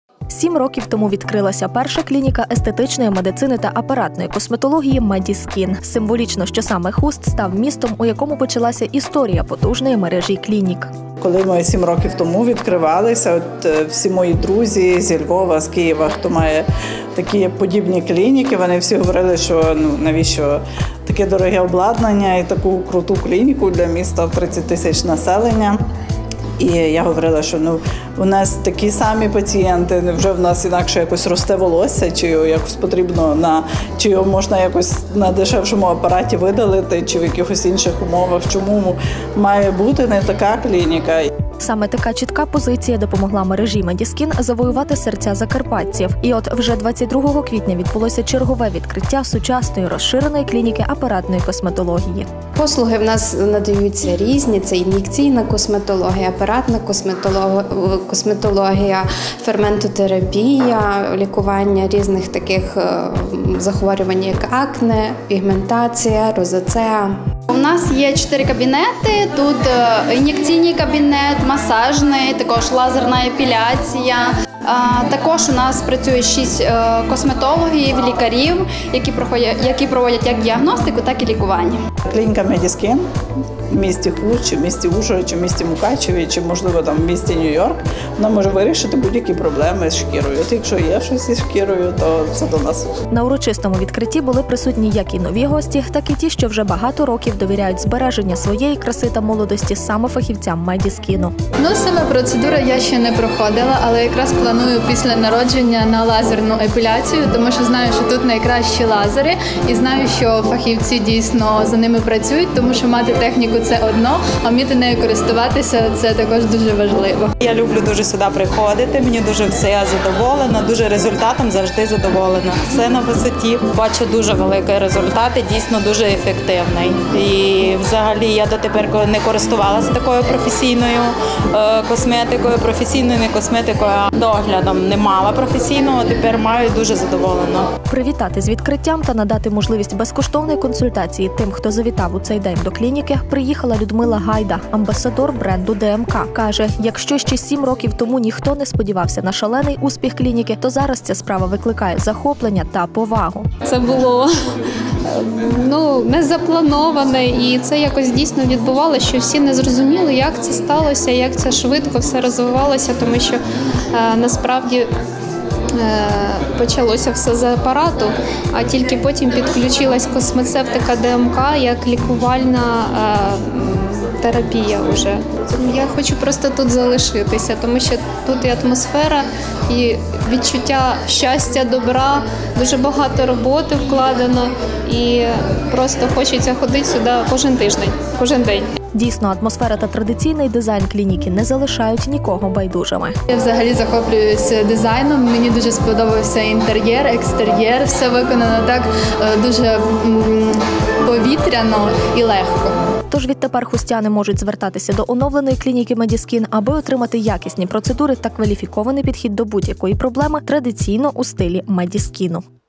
Відкриття клініки «MediSkin». Естетична медицина та апаратна косметологія
22 квітня відбулося урочисте відкриття клініки Медіскін у Хусті. Медіскін тут існує вже 7 років. 7 років тому відкрилася перша клініка естетичної медицини та апаратної косметології Медіскін. Символічно, що саме Хуст став містом, у якому почалася історія потужної мережі клінік. Саме така чітка позиція допомогла мережі «МедіСкін» завоювати серця закарпатців.